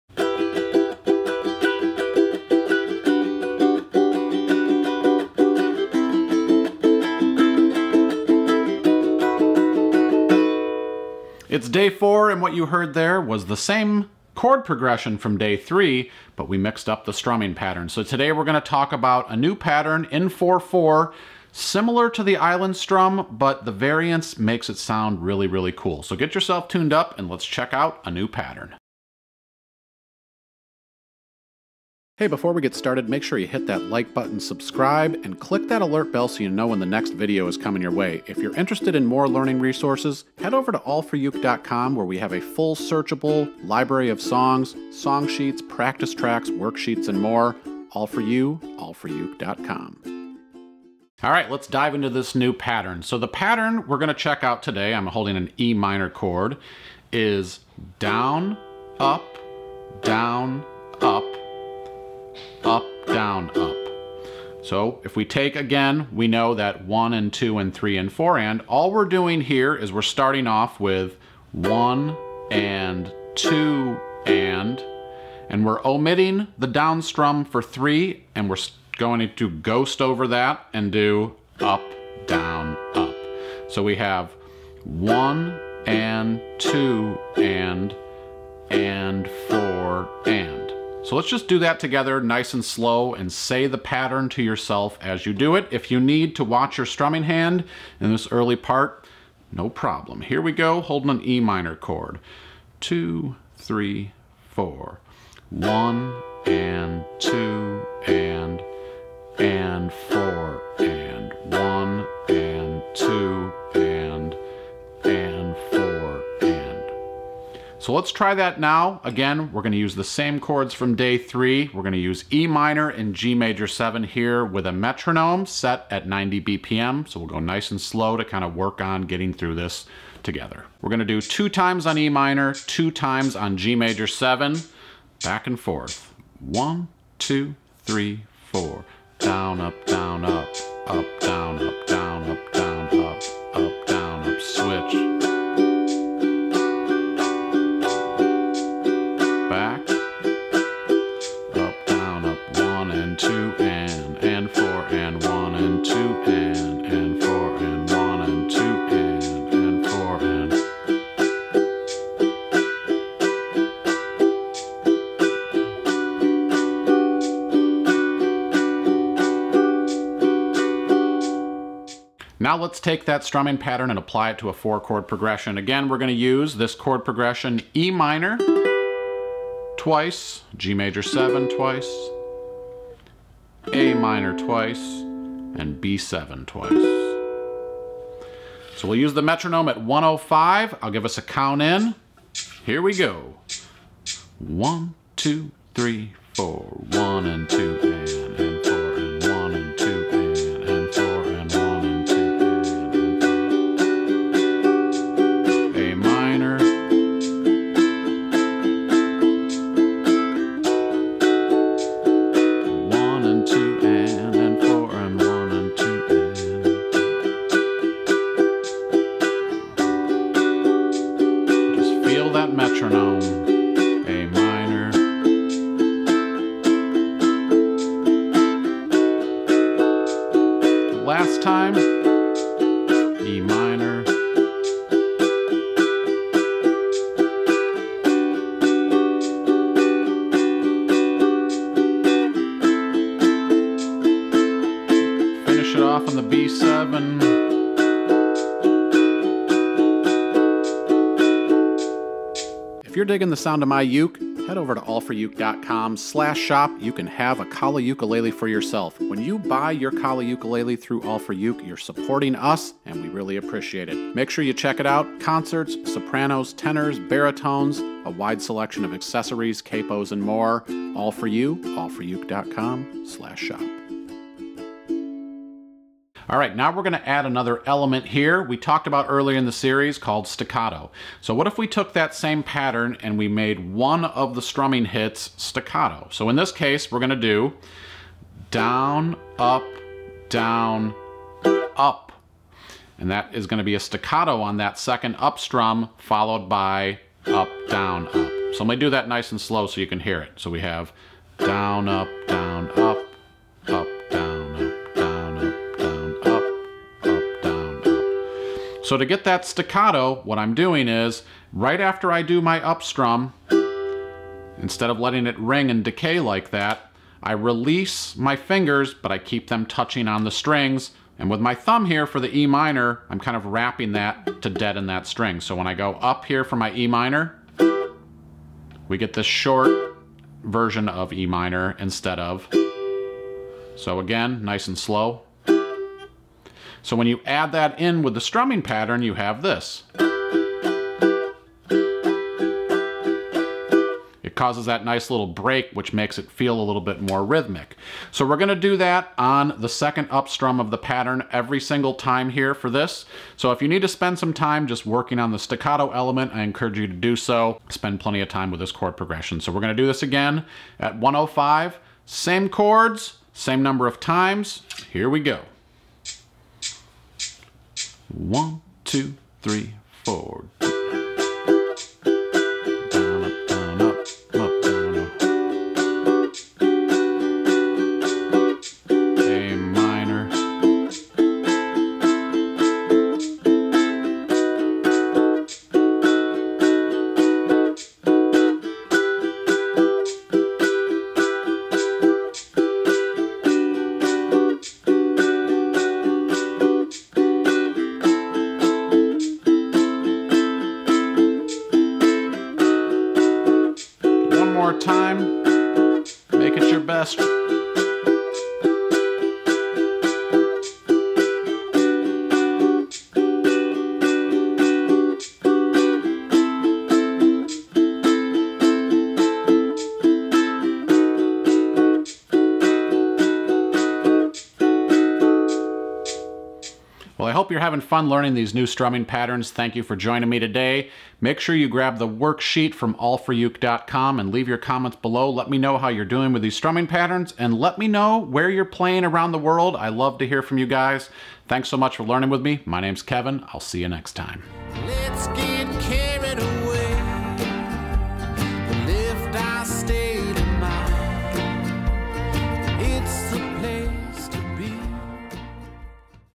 Step Up Your Strumming | Level Two | Day 4 | Ukulele Tutorial + Play Along
Step Up Your Strumming Level 2 has arrived! In this series, we'll explore variations of ukulele strumming patterns we learned in level one as well as new patterns and techniques such as muting/chucking and staccato. We'll also spend some time learning how to work with a metronome to really build your timing skills. Don't forget to grab your free worksheet and let's have some fun with LEVEL TWO! - Kevin

♥︎ M E M B E R W O R K S H E E T

✮ Z O O M W I T H K E V I N

★ D I G M Y K A L A U K E ?

✮ F B G R O U P I N S T A @AllForUke

▷   V I D E O  M E N T I O N S